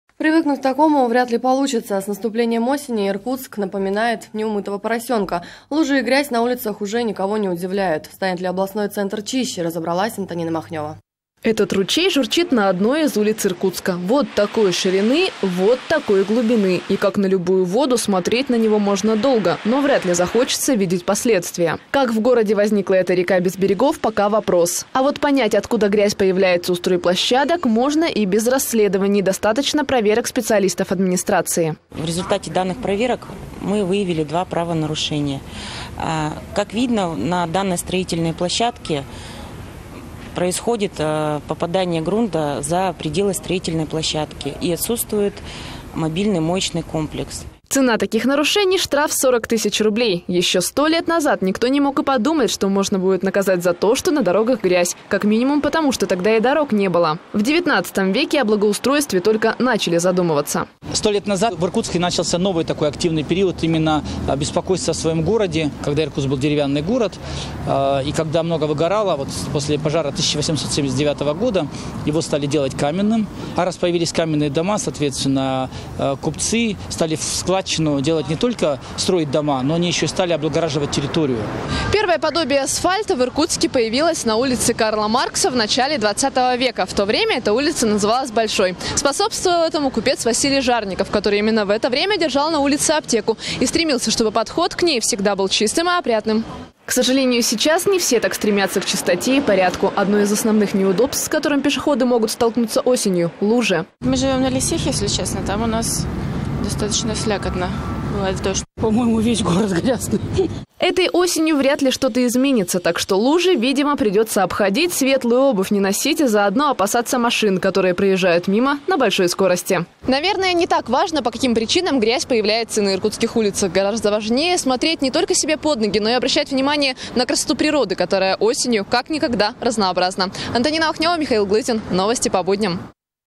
История иркутской уличной грязи
Привыкнуть к такому вряд ли получится. С наступлением осени Иркутск напоминает неумытого поросенка. Лужи и грязь на улицах уже никого не удивляют. Станет ли областной центр чище, разобралась Антонина Махнёва. Этот ручей журчит на одной из улиц Иркутска. Вот такой ширины, вот такой глубины. Как и на любую воду, смотреть на него можно долго. Но навряд ли, захочется видеть последствия. Как в городе возникла эта река без берегов, пока вопрос. А вот чтобы понять, откуда грязь появляется у стройплощадок, можно догадаться и без расследований. Достаточно проверок специалистов администрации. АНАСТАСИЯ ШАБУНОВА, ответственный секретарь административной комиссии: «В результате проверок мы выявили два правонарушения. На данной площадке происходит попадание грунта за пределы строительной площадки, и отсутствует мобильный моечный комплекс». Цена таких нарушений - штраф 40 тысяч рублей. Еще сто лет назад никто не мог и подумать, что можно наказать за то, что на дорогах грязь. Как минимум, потому, что тогда и дорог не было. В 19 веке о благоустройстве только начали задумываться. АЛЕКСЕЙ ПЕТРОВ, историк: «Это было связано с известными пожарами, когда Иркутск был деревянным городом. После пожара в 1879 году Иркутск стали делать каменным. А раз появились каменные дома, купцы стали в складчину облагораживать территории возле своих домов». АНТОНИНА МАХНЁВА, корреспондент: «Первое подобие асфальта в Иркутске появилось на улице Карла Маркса в начале двадцатого века. Тогда эта улица называлась Большой. Способствовал этому купец Василий Жарников, который именно в это время держал на улице аптеку и стремился, чтобы подход к ней был чистым и опрятным». К сожалению, сейчас не все так стремятся к чистоте и порядку. Одно из основных неудобств, с которым пешеходы могут столкнуться осенью, - лужи. Этой осенью вряд ли что-то изменится. Так что, лужи, видимо, придется обходить, светлую обувь не носить, а заодно опасаться машин, которые проезжают мимо на большой скорости. Наверное, не так важно, по каким причинам грязь появляется на улицах города. Гораздо важнее смотреть не только себе под ноги, но и обращать внимание на красоту природы, которая осенью как никогда разнообразна.